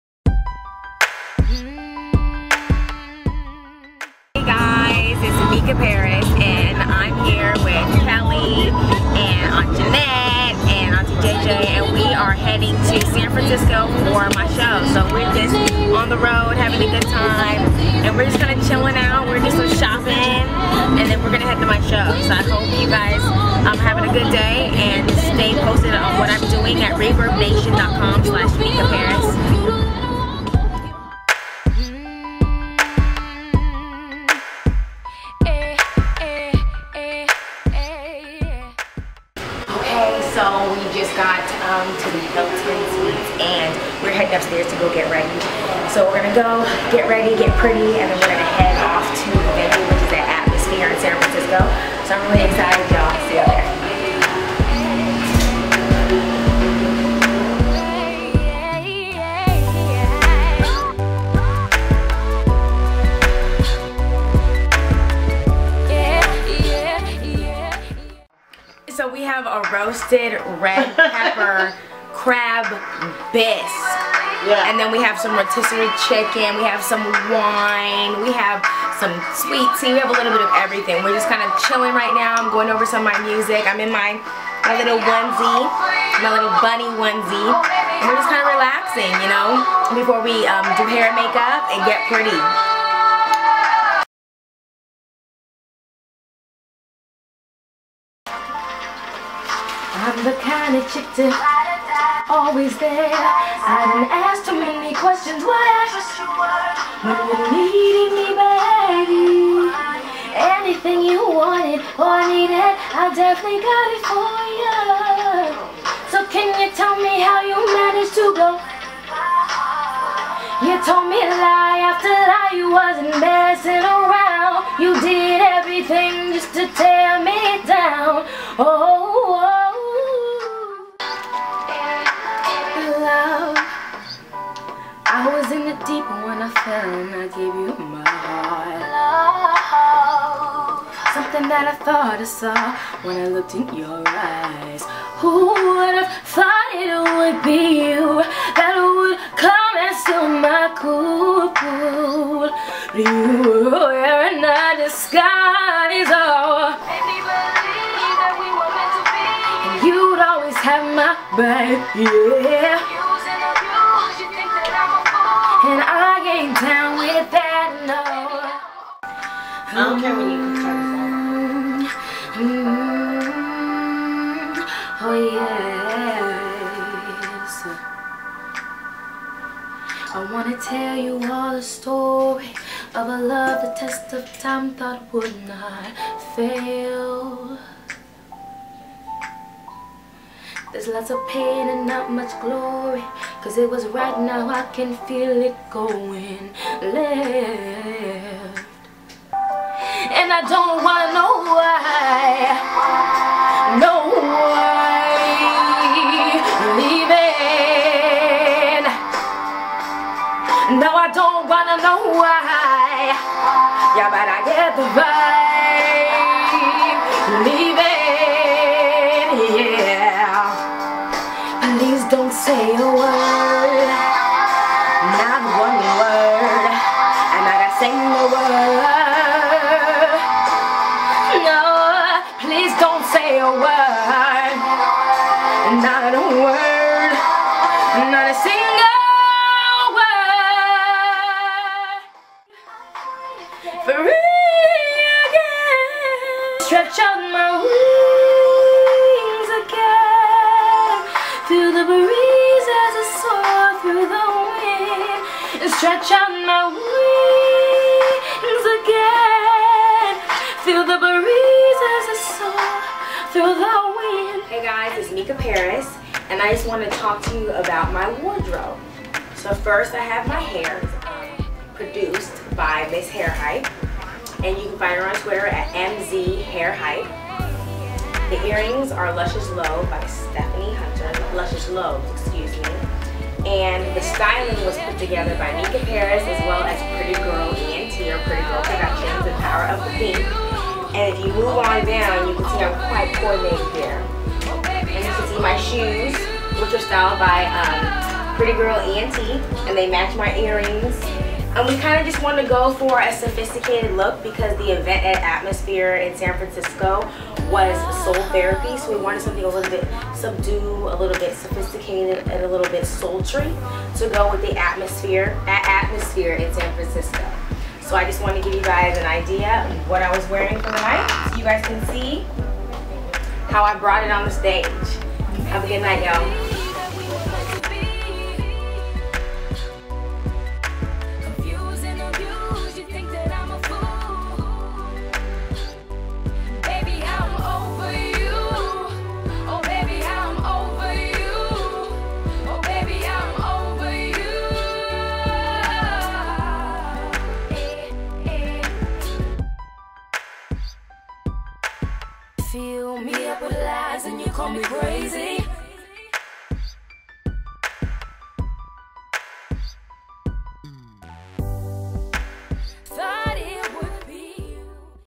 Mica Parris Behind the Scenes - San Francisco *Pretty Girls Ent.*
Pretty Girls ENT., Kelli B. Photo & Mica Parris take you behind the scenes footage of Mica Parris on the road, rehearsing in her hotel, and her wardrobe for her show in San Francisco.